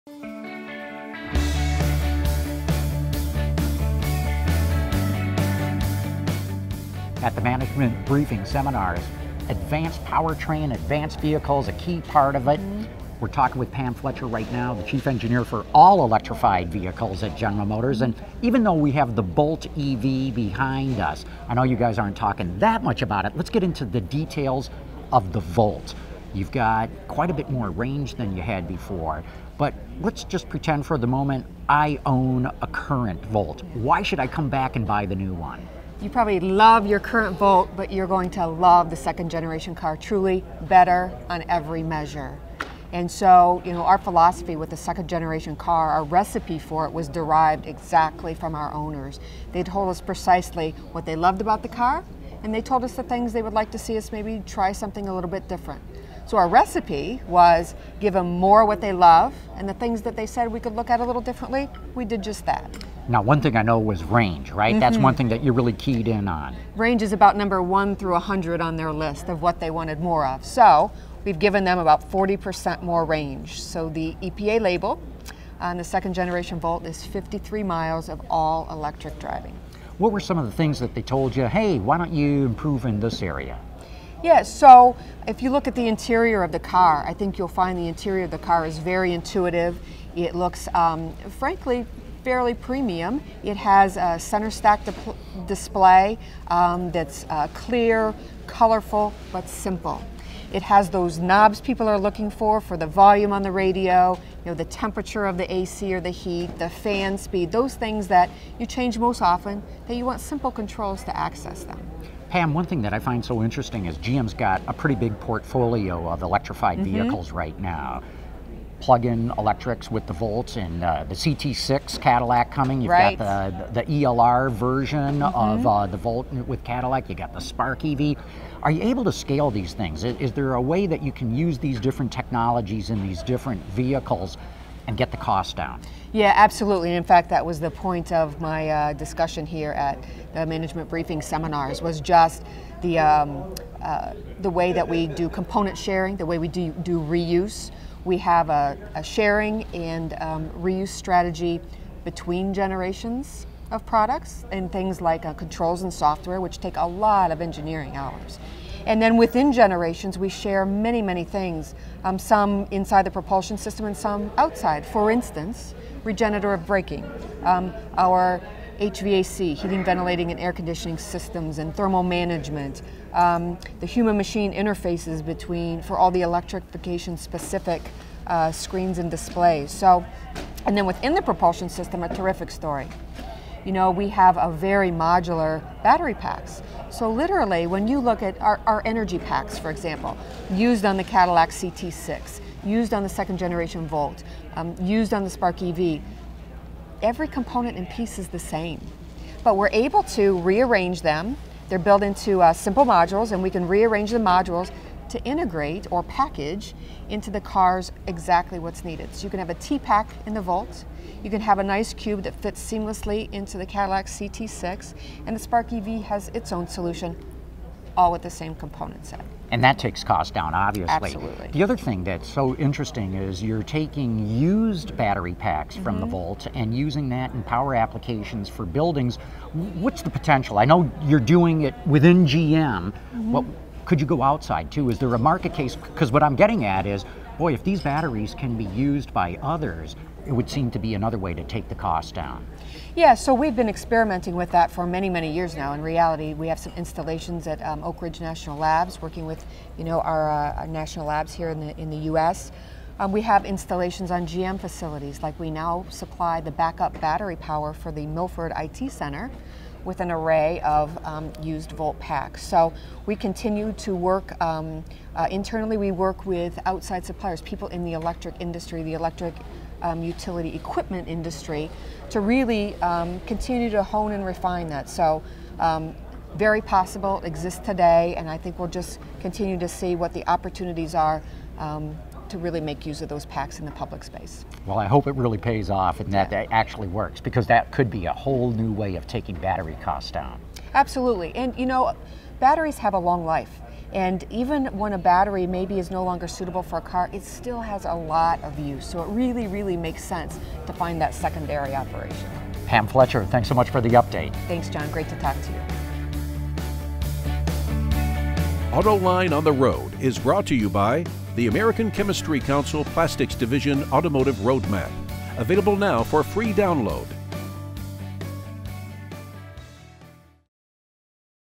GM’s EV Batteries Get Second Life - CAR MBS 2015
Pam Fletcher is the Executive Chief Engineer of Electrified Vehicles at General Motors. She talks with John McElroy about whether enough has improved in the 2016 Chevrolet Volt that current owners will want to upgrade. Fletcher also discusses the features drivers requested and how expired batteries are finding a use outside of cars.